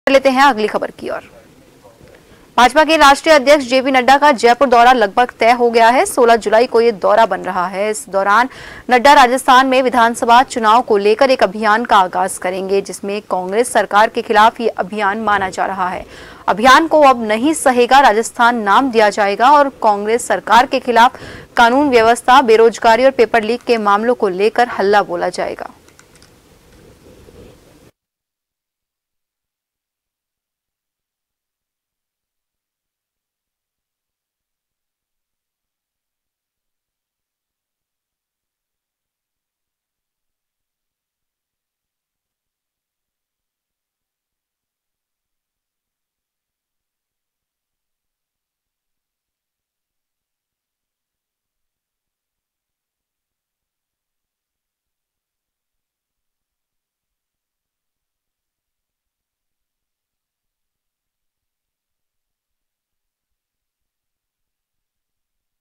BJP के राष्ट्रीय अध्यक्ष JP Nadda का Jaipur दौरा, Congress सरकार के खिलाफ अभियान का करेंगे आगाज
NEWS CREDIT-GS